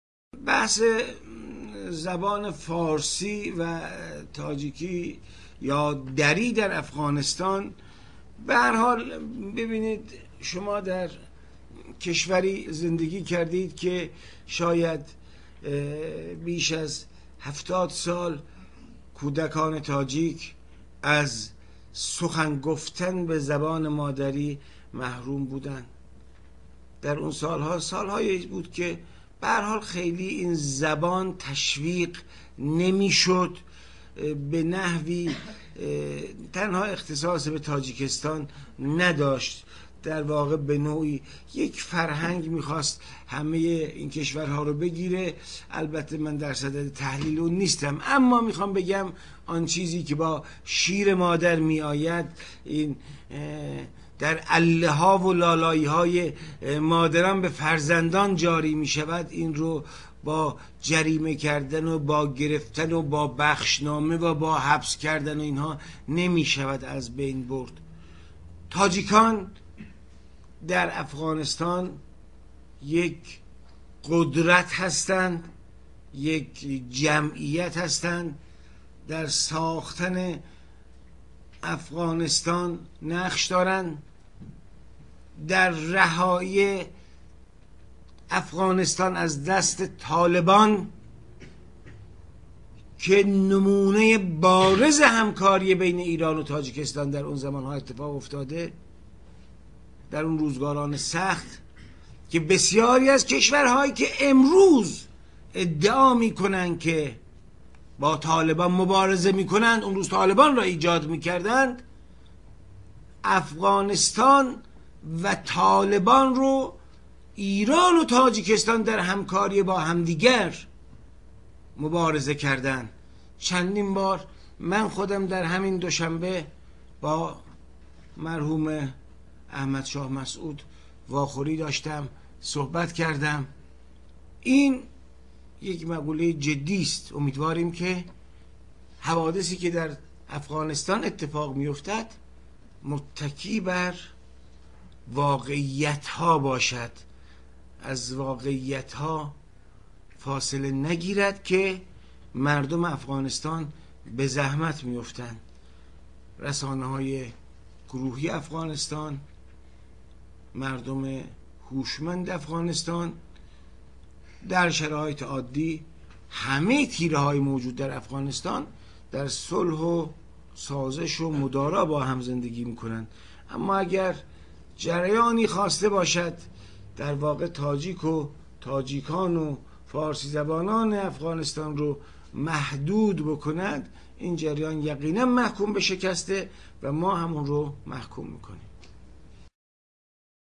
Ali Askar Sherdust, respond to Khuram Khugzai Mordagawwals
Ali Askar Sherdust, Iran's Ambassador to Tajikistan Responds to Khurram Khugzai Mordagawwal´s Decisions banning Persian language in Afghanistan and making Persian-Speaking and ethnic Persians (Tajiks) of Awghanistan to cultureless, wild, animalic, savage, uncivilizised, nomadic, homosexual, sodomy practizing, backward PigToons/FuckToons, 19 Feb 2008